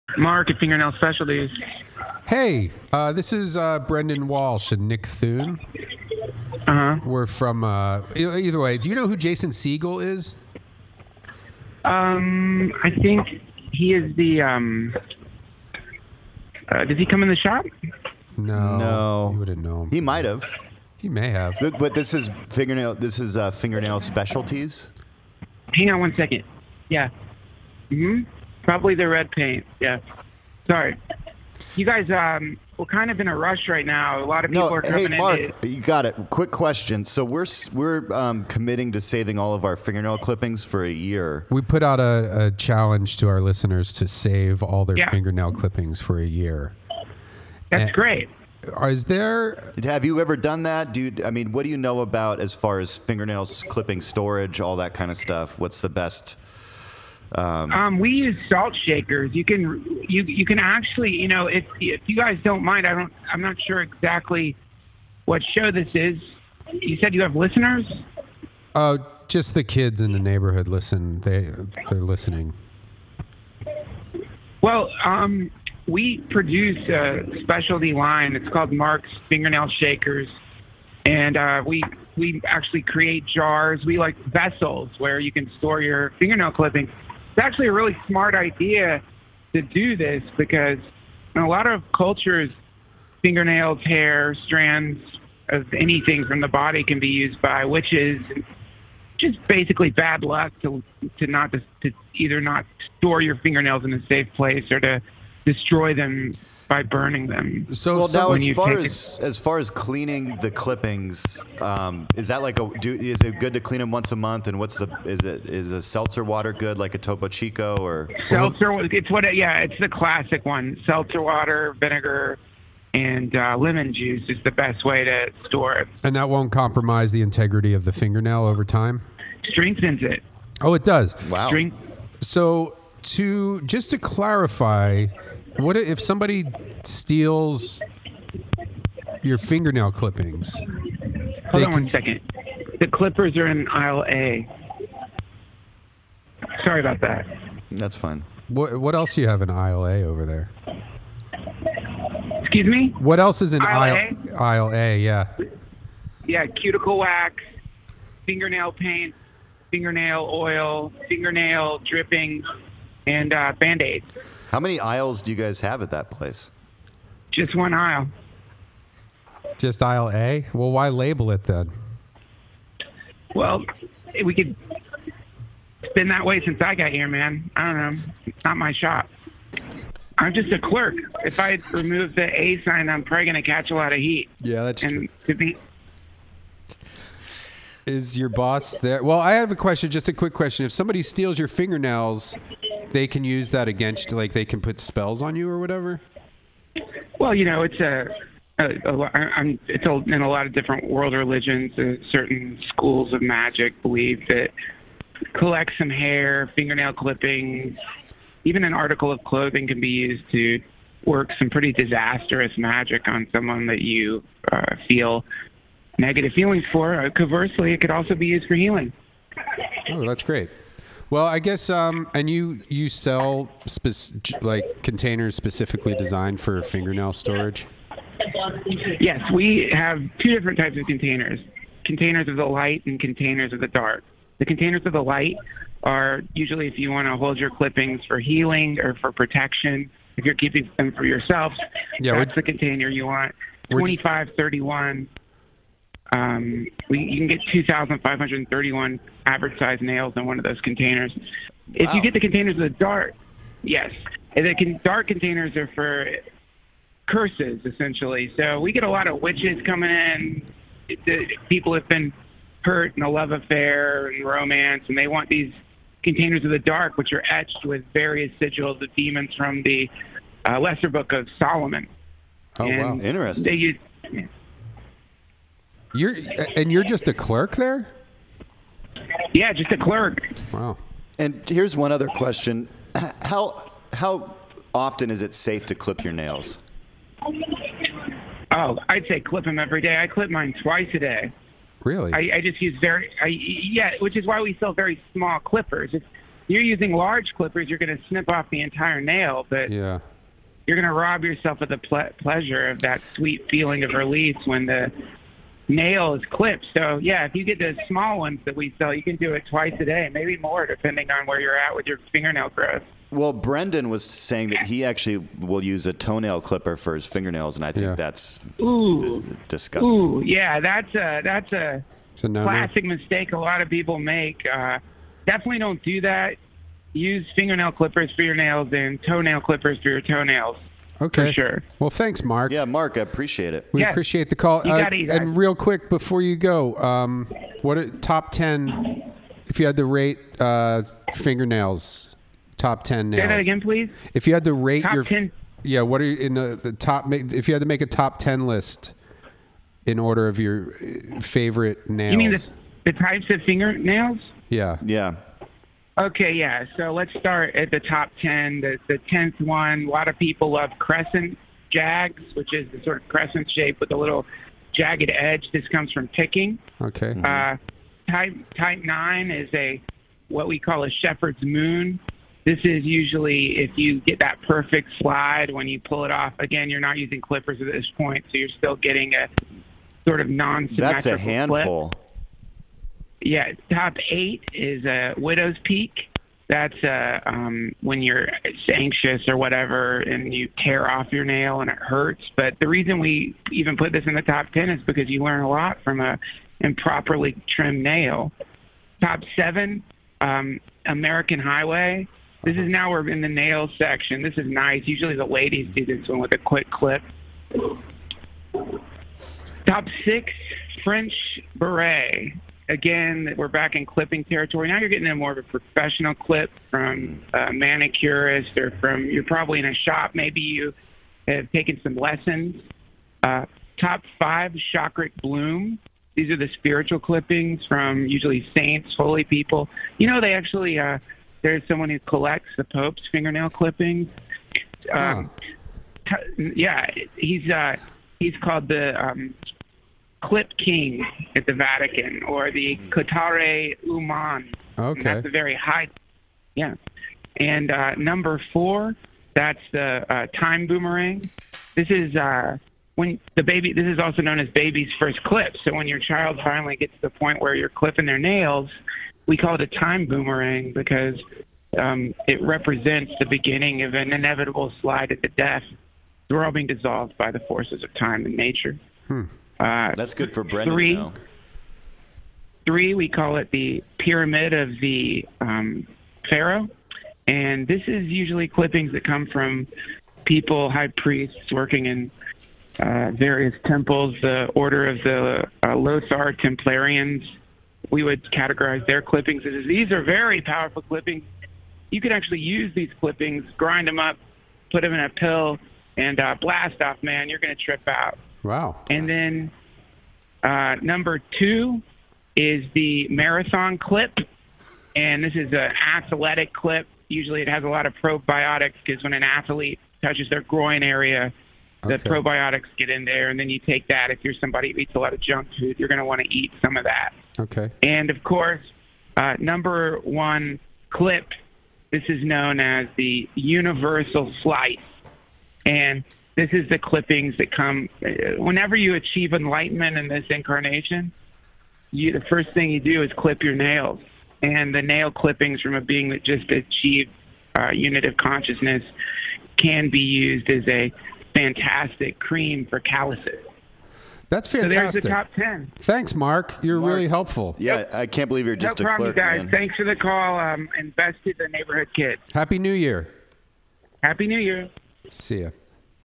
MARK FROM FINGERNAIL SPECIALTIES
Brendon and Nick talk to Mark from fingernail specialties and get educated on the different kinds of fingernails. Taken from Episode 14: Washington

Listen to the podcast at AllThingsComedy.com
Follow Brendon Walsh on Twitter @BrendonWalsh
Follow Nick Thune on Twitter @NickThune